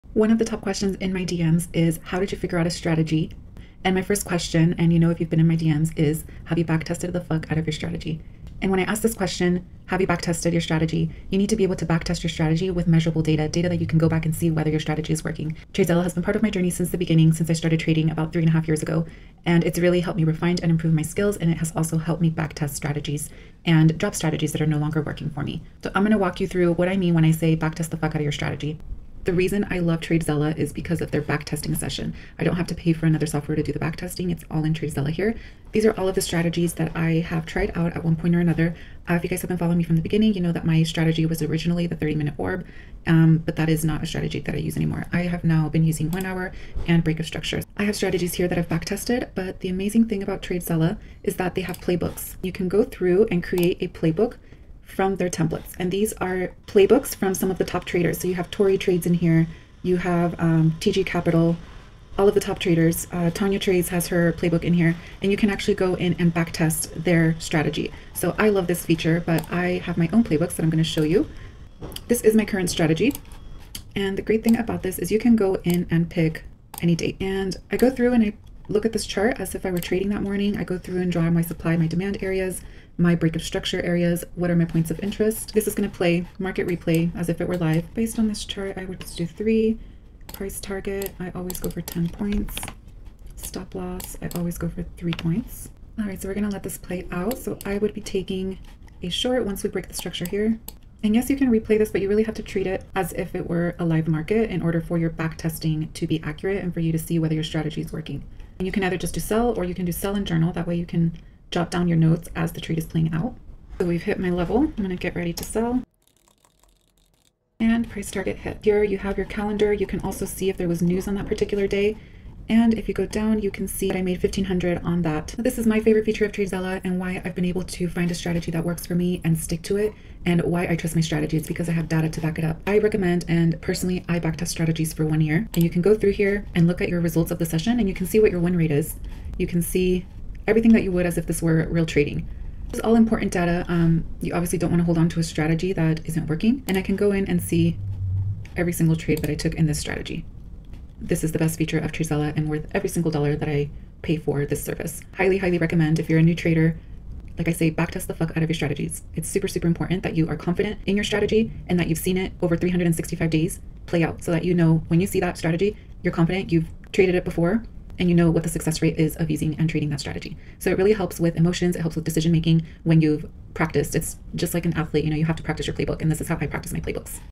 How I Built Confidence in My Trading Strategy Using Data (TradeZella Walkthrough)
I don’t have a “secret” strategy. I have data.
Backtesting with TradeZella showed me what actually works — and that’s where confidence comes from.